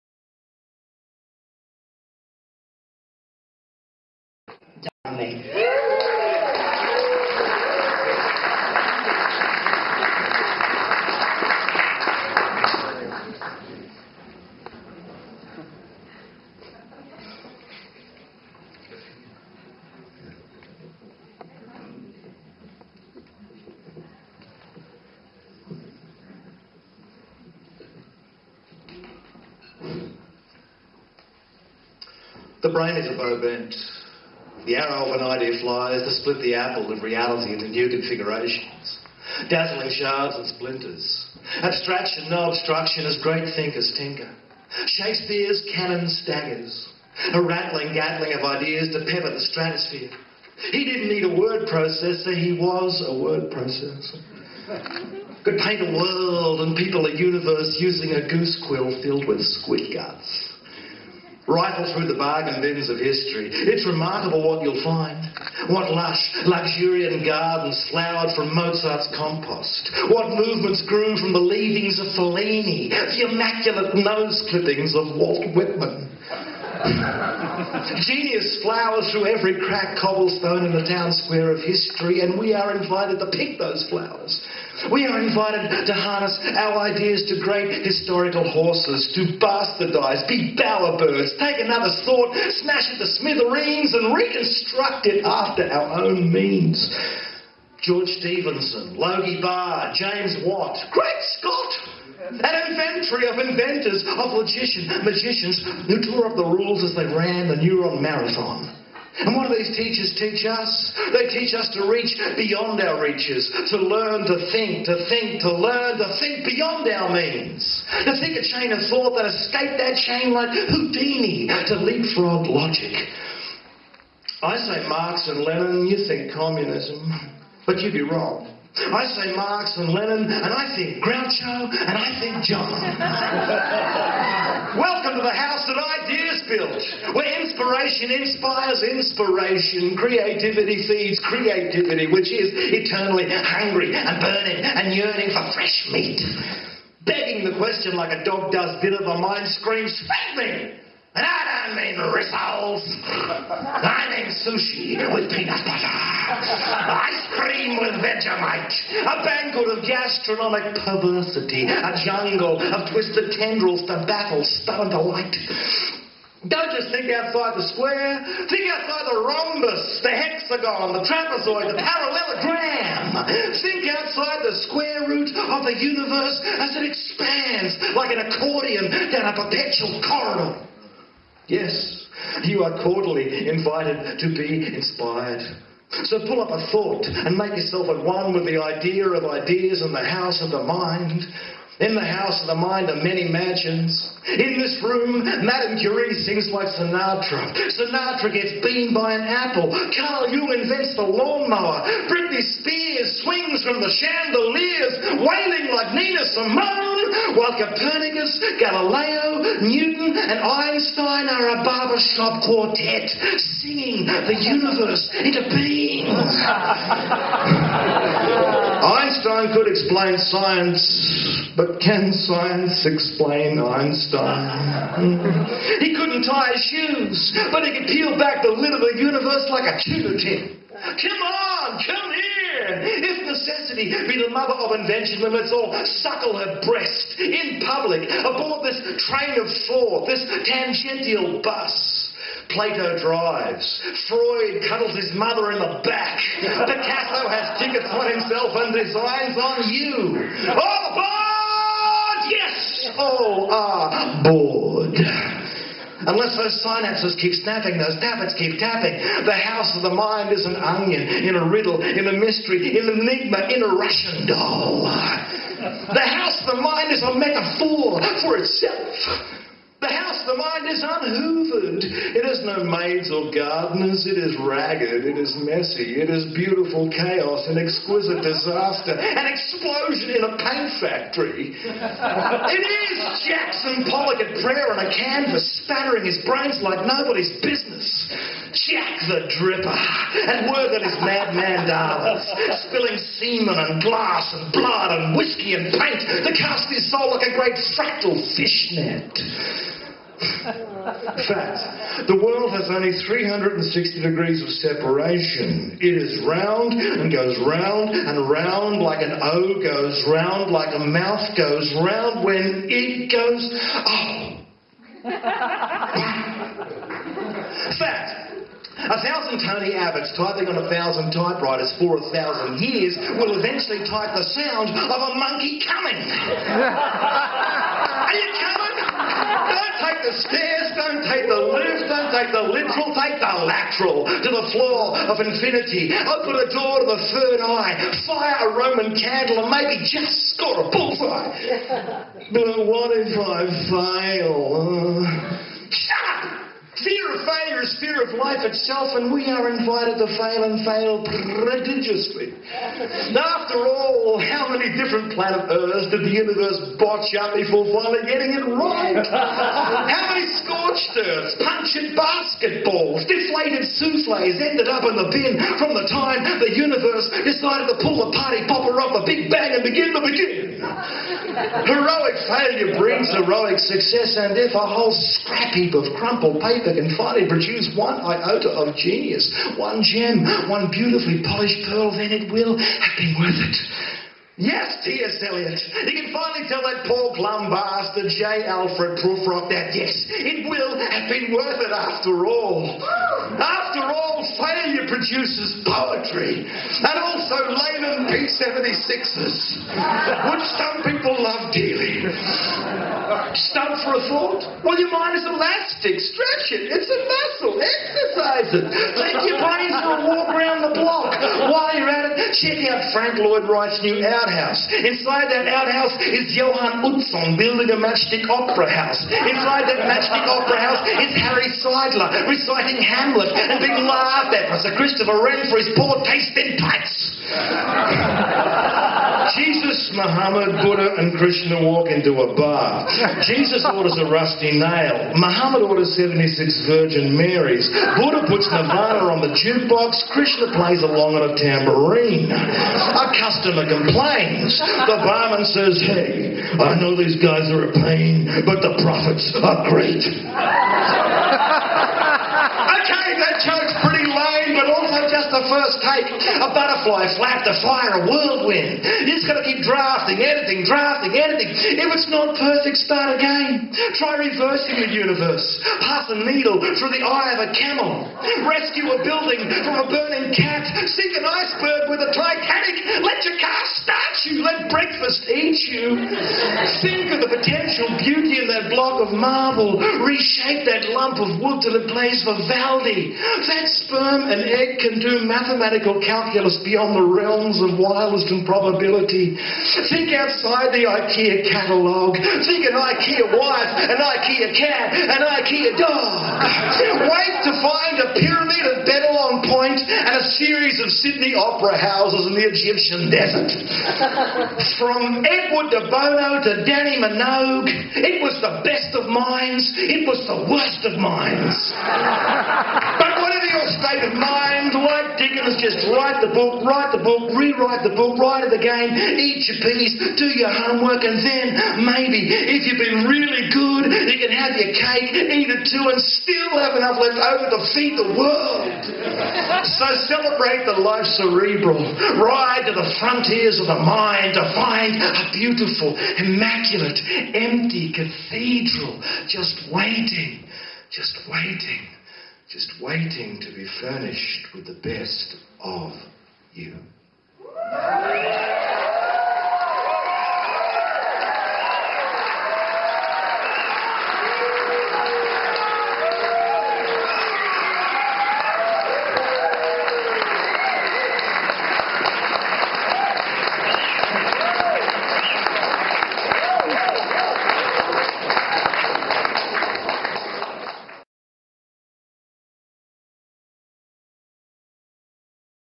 Tug Dumbly 2010 NPPWC Nimbin Poetry
Eight performers from the Heats and Semi-Finals qualify for the Finals of the Nimbin Performance Poetry World Cup (NPPWC) held on the first weekend of August each year and performed live on the stage of the Nimbin Town Hall.